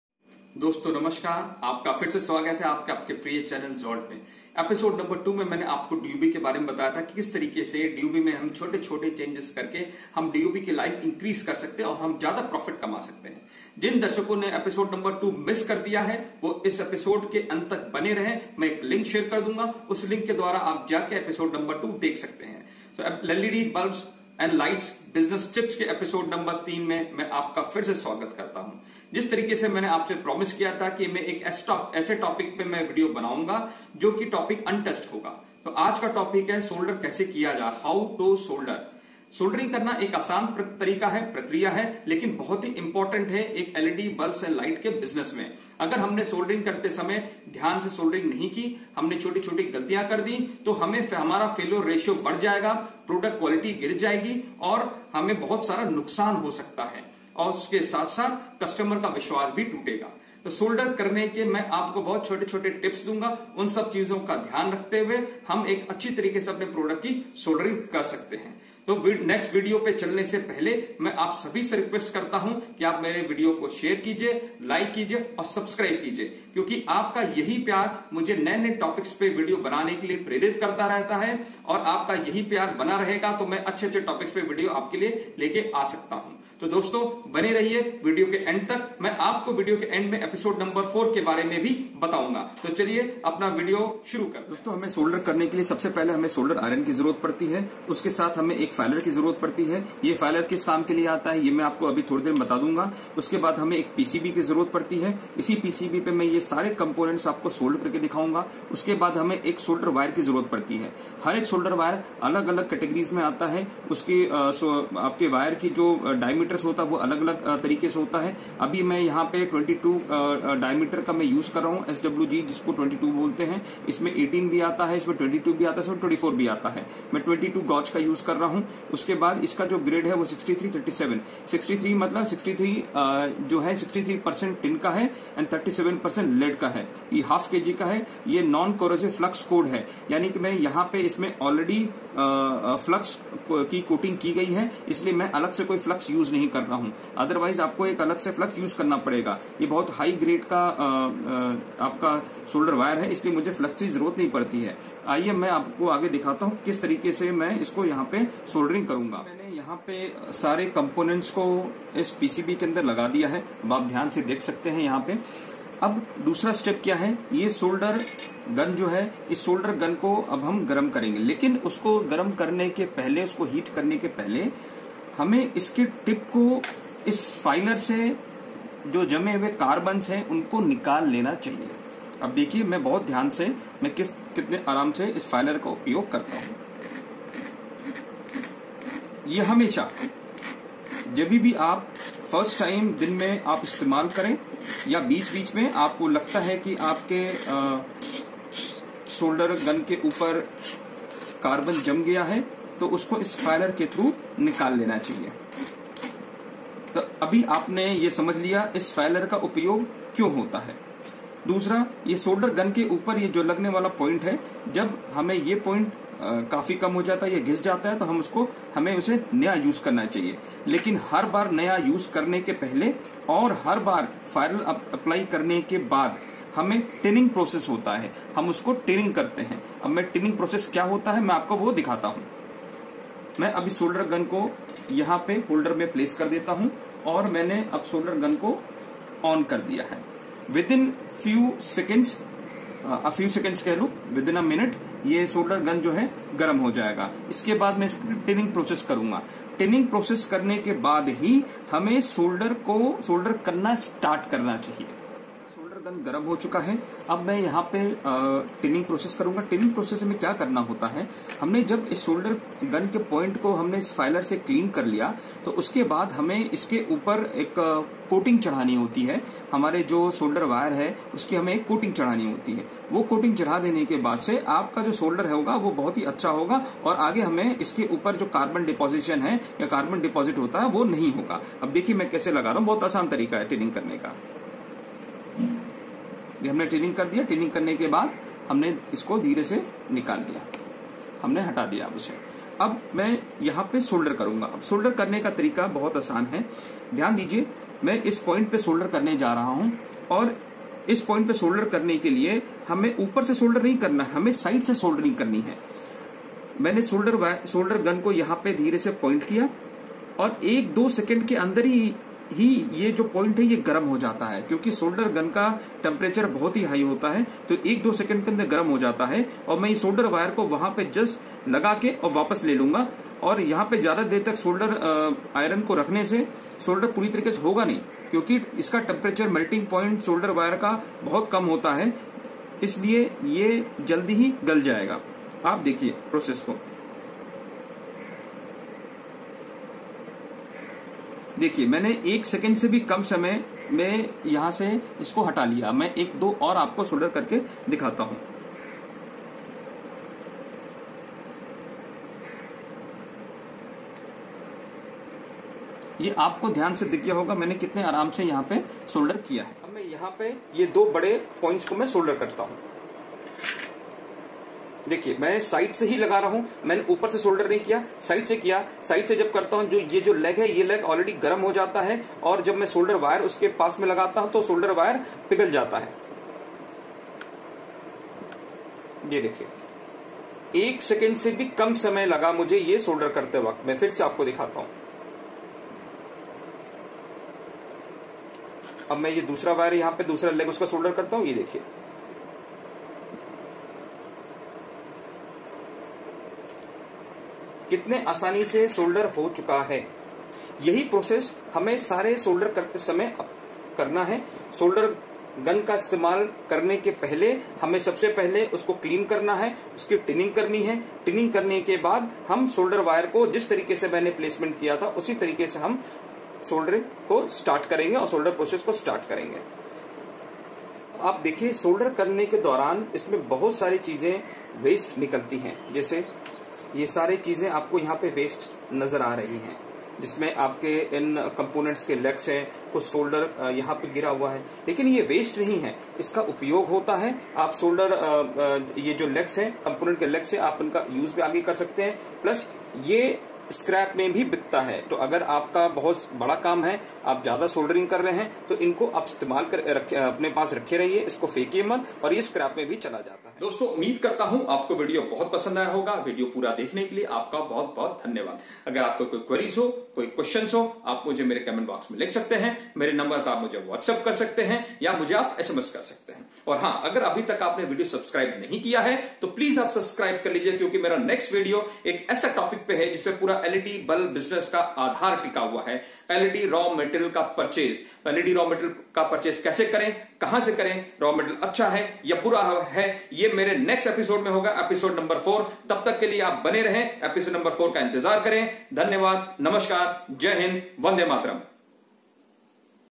सोल्डर करना सीखें मात्र 5 मिनट में। Learn SOLDERING in 5 min & LED BULB SOLDER का सही तरीका।
This video is about, how to do Soldering. Soldering is very important in LED Bulb Manufacturing Process. A good solder will reduce the failure of the product and resulting into High Profitability. Some of the basic steps should be followed while starting any soldering process.
1. Solder tips should be properly cleaned and all carbon / residues should be removed with the help of filer.
2. Once solder tip is cleaned, Tinning process should be performed.
3. Use good quality of Solder Wire with Flux core.
4. Use good quality of Solder iron.
We have seen that good soldering process reduce atleast 20% of failure and approx 15% of repeated work.

LED BULB निर्माण प्रक्रिया में SOLDER बहुत महत्वपूर्ण है। एक अच्छा soldering उत्पाद की विफलता को कम करेगा और जिसके परिणामस्वरूप उच्च लाभप्रदता होगी। किसी भी SOLDER प्रक्रिया शुरू करते समय कुछ बुनियादी चरणों का पालन किया जाना चाहिए।
1. SOLDER Tips को ठीक से साफ किया जाना चाहिए और सभी कार्बन/अवशेषों को फाइलर की मदद से हटाया जाना चाहिए ।
2. एक बार SOLDER Tips साफ हो जाने के बाद, टिनिंग प्रक्रिया की जानी चाहिए।
3. फ्लक्स कोर के साथ SOLDER WIRE की अच्छी गुणवत्ता का उपयोग करें।
4. Solder Iron अच्छी गुणवत्ता का उपयोग करें